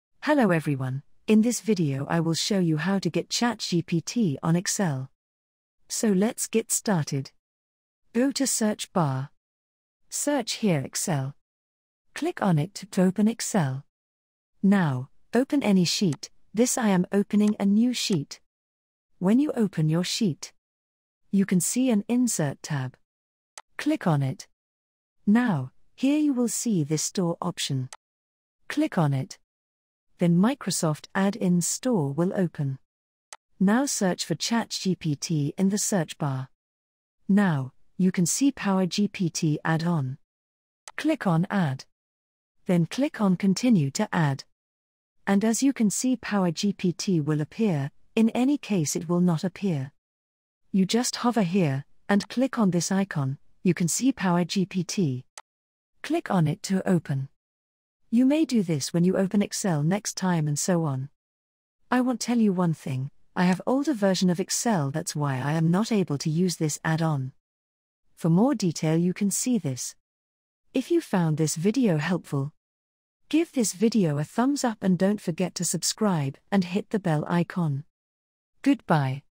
How To Use AI In Excel Sheet | Get ChatGPT For Excel Sheet
Learn how to use AI in Excel Sheet to enhance your productivity and automate tasks. This tutorial will show you how to integrate AI tools like ChatGPT into your Excel workflow, making data analysis and report generation easier and more efficient.